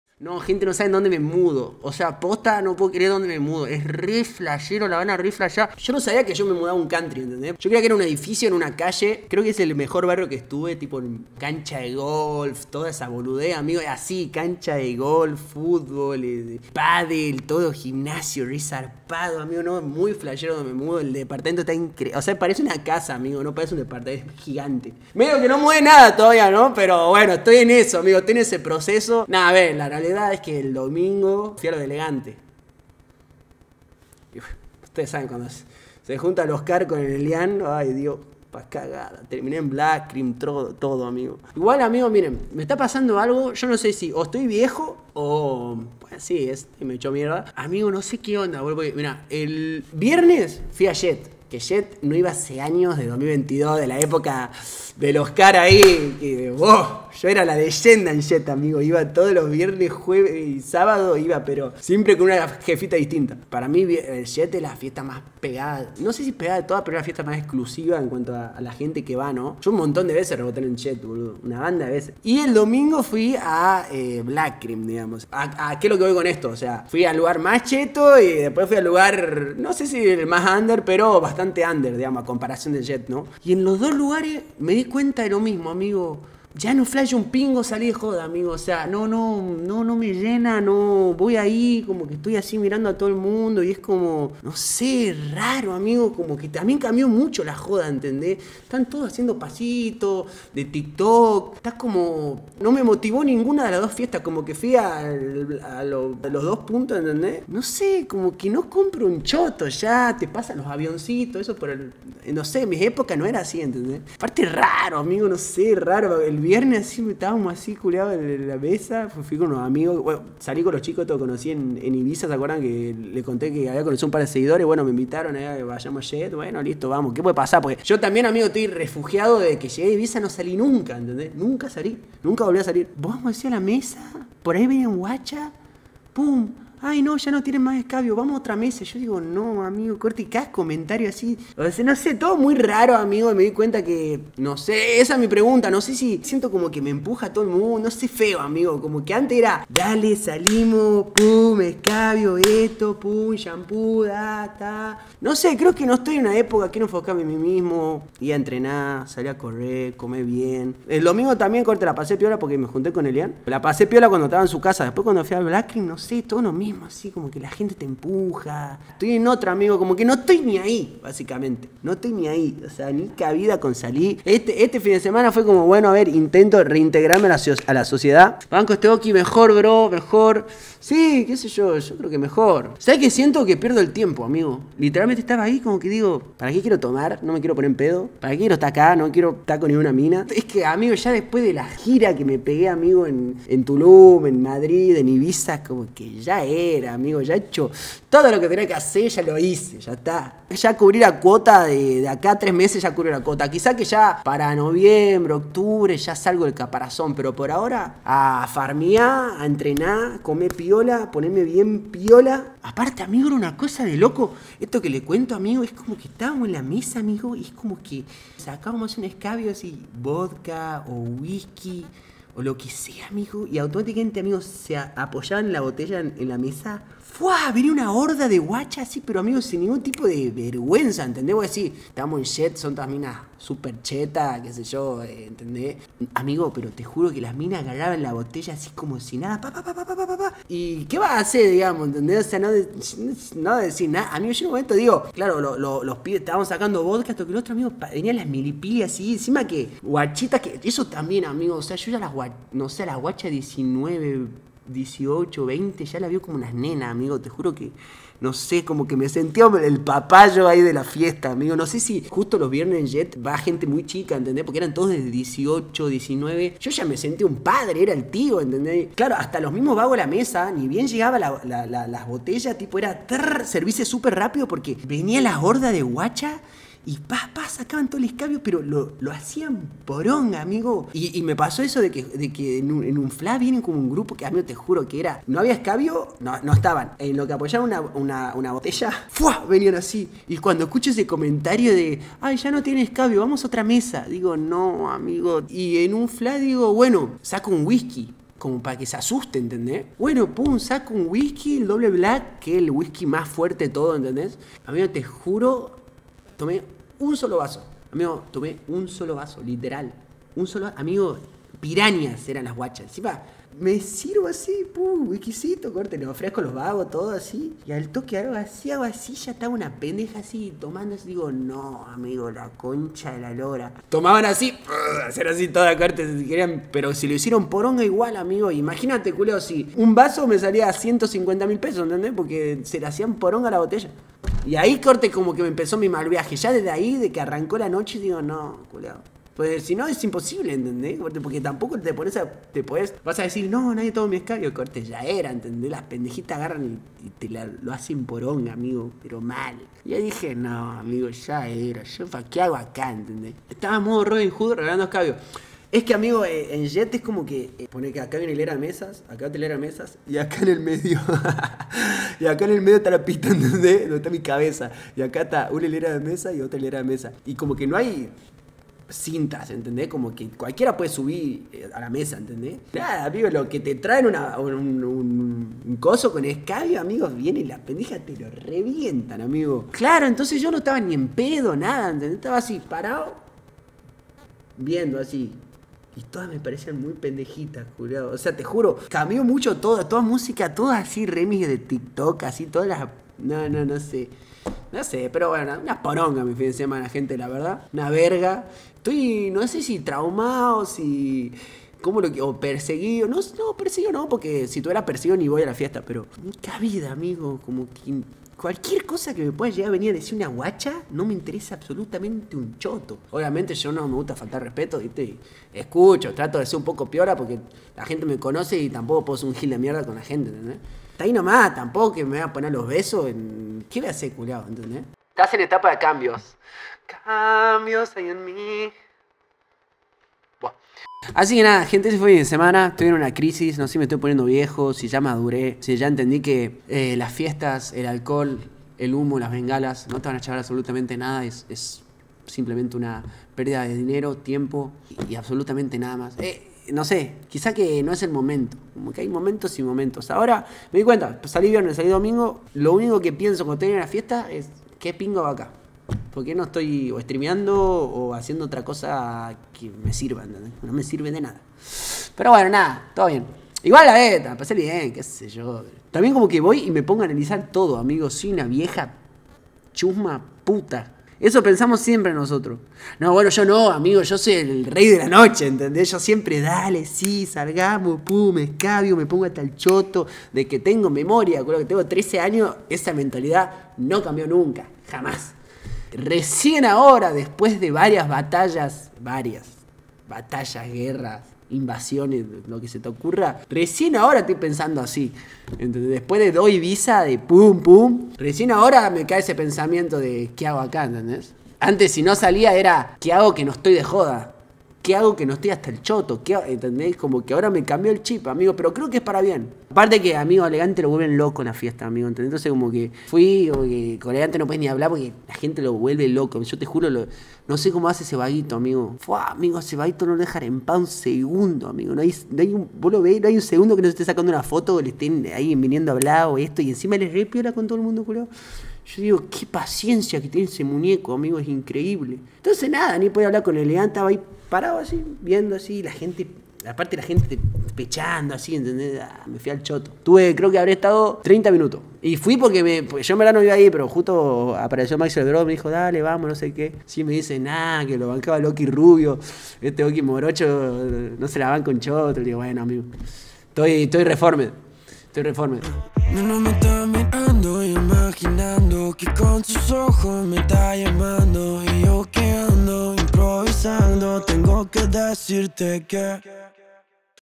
VOLVÍ a SALIR de JODA con L-GANTE ¿Ya ESTOY VIEJO para SALIR?
Volví a las pistas pero la fiesta ya no es lo que era.